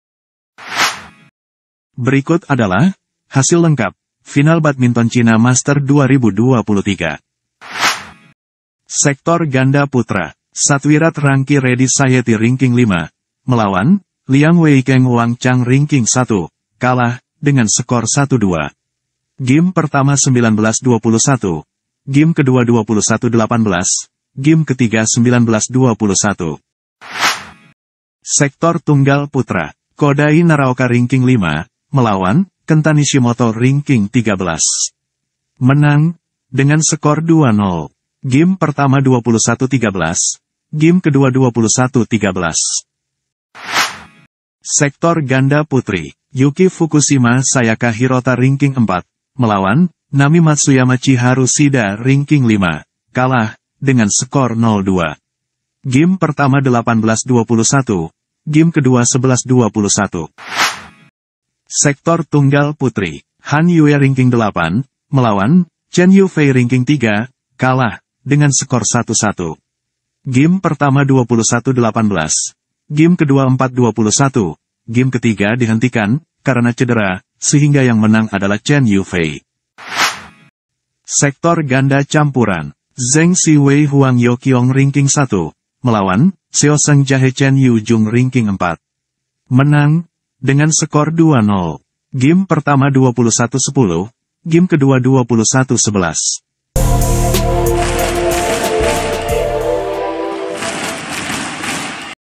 Hasil Lengkap Final china Master 2023 Hari ini | Indonesia Tanpa gelar ..!!
Hasil Lengkap Final china Master 2023 Hari ini | KENG/WANG & KODEI NARAOKA & N MTSYMA/C SHIDA & CHEN Y FEI & ZHANG/QIONG Juara.!!